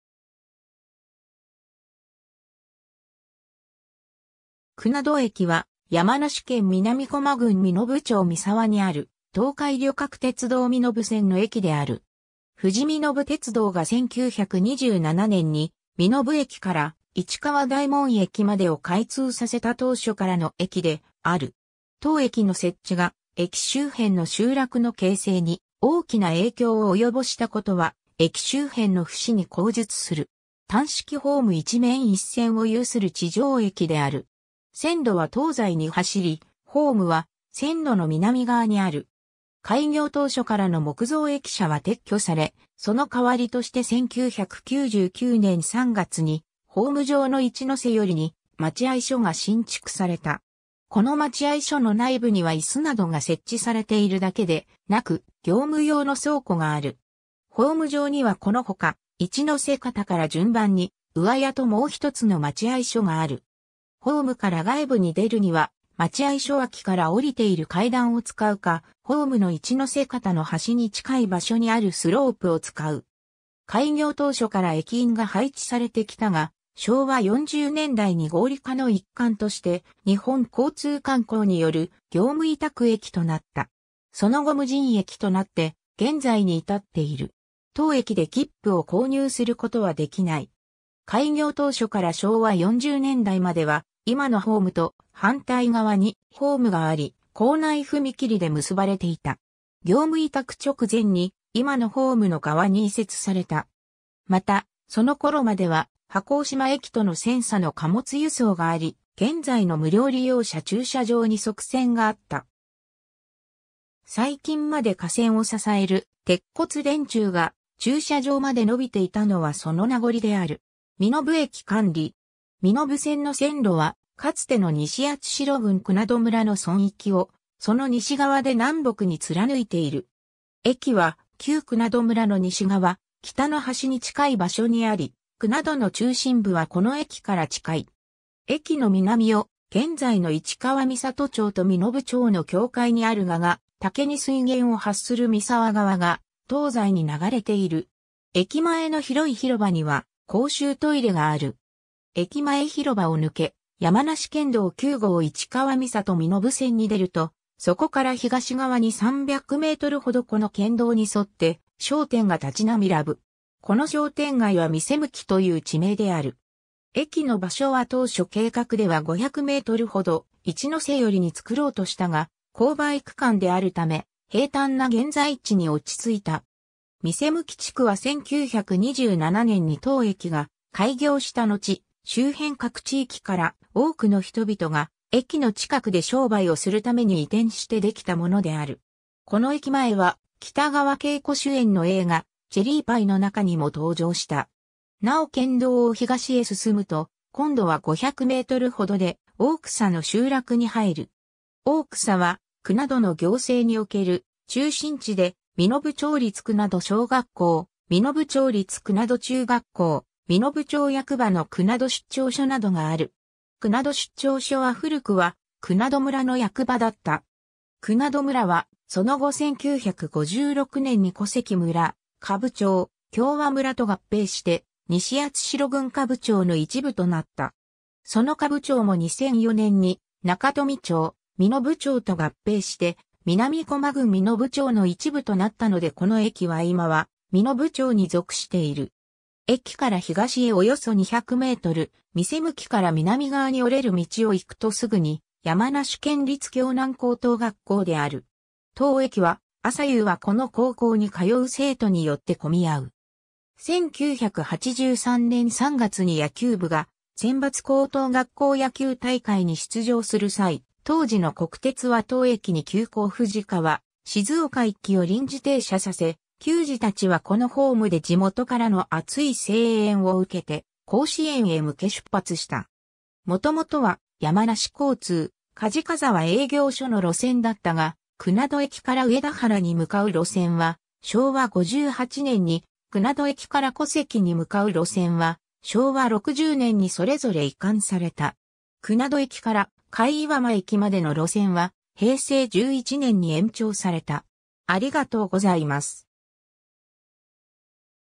久那土駅
久那土駅（くなどえき）は、山梨県南巨摩郡身延町三澤にある、東海旅客鉄道（JR東海）身延線の駅である。
 富士身延鉄道が1927年（昭和2年）に身延駅から市川大門駅までを開通させた当初からの駅である。
当駅の設置が駅周辺の集落の形成に大きな影響を及ぼしたことは駅周辺の節に後述する。
 単式ホーム1面1線を有する地上駅である。
線路は東西に走り、ホームは線路の南側にある。
 開業当初からの木造駅舎は撤去され、その代わりとして1999年（平成11年）3月に、ホーム上の市ノ瀬寄りに待合所が新築された。
この待合所の内部にはいすなどが設置されているだけでなく、業務用の倉庫がある。
ホーム上にはこのほか、市ノ瀬方から順番に上屋ともう一つの待合所がある。
ホームから外部に出るには待合所脇から降りている階段を使うか、ホームの市ノ瀬方の端に近い場所にあるスロープを使う。
 開業当初から駅員が配置されてきたが、昭和40年代に合理化の一環として日本交通観光による業務委託駅となった。
その後無人駅となって現在に至っている。
当駅で切符を購入することはできない。
 開業当初から昭和40年代までは今のホームと反対側にホームがあり、構内踏切で結ばれていた。
業務委託直前に今のホームの側に移設された。
また、その頃までは波高島駅との川砂の貨物輸送があり、現在の無料利用者駐車場に側線があった。
最近まで架線を支える鉄骨電柱が駐車場まで伸びていたのはその名残である。
 身延駅管理。
 身延線の線路は、かつての西八代郡久那土村の村域を、その西側で南北に貫いている。
駅は旧久那土村の西側、北の端に近い場所にあり、久那土の中心部はこの駅から近い。
駅の南を現在の市川三郷町と身延町の境界にある蛾ヶ岳（標高1280メートル）に水源を発する三沢川が東西に流れている。
 駅前の広い広場には公衆トイレがある。
駅前広場を抜け、山梨県道9号市川三郷身延線に出ると、そこから東側に300メートルほどこの県道に沿って商店が立ち並らぶ。
この商店街は店向（三澤11組）という地名である。
駅の場所は当初計画では500メートルほど市ノ瀬寄りに作ろうとしたが、勾配区間であるため、平坦な現在地に落ち着いた。
店向地区は1927年（昭和2年）に当駅が開業したのち、周辺各地域から多くの人々が駅の近くで商売をするために移転して出来たものである。
 この駅前は、北川景子主演の映画「チェリーパイ 」の中にも登場した。
 なお県道を東へ進むと今度は500メートルほどで大草の集落に入る。
大草は久那土の行政における中心地で、身延町立久那土小学校、身延町立久那土中学校、身延町役場の久那土出張所などがある。
久那土出張所は古くは久那土村の役場だった。
 久那土村はその後1956年（昭和31年）に古関村、下部町、共和村と合併して西八代郡下部町の一部となった。
その下部町も2004年（平成16年）に中富町、身延町と合併して南巨摩郡身延町の一部と成ったのでこの駅は今は身延町に属している。
 駅から東へおよそ200メートル、店向から南側に折れる道を行くとすぐに山梨県立峡南高等学校である。
当駅は朝夕はこの高校に通う生徒によって込み合う。
1983年（昭和58年）3月に野球部が選抜高等学校野球大会に出場する際、当時の国鉄は当駅に急行富士川・静岡行きを臨時停車させ、球児たちはこのホームで地元からの熱い声援を受けて甲子園へ向け出発した。
 もともとは山梨交通㈱鰍沢営業所の路線だったが、久那土駅から上田原に向かう路線は昭和58年に、久那土駅から古関に向かう路線は昭和60年にそれぞれ移管された。
久那土駅から甲斐岩間駅までの路線は平成11年に延長された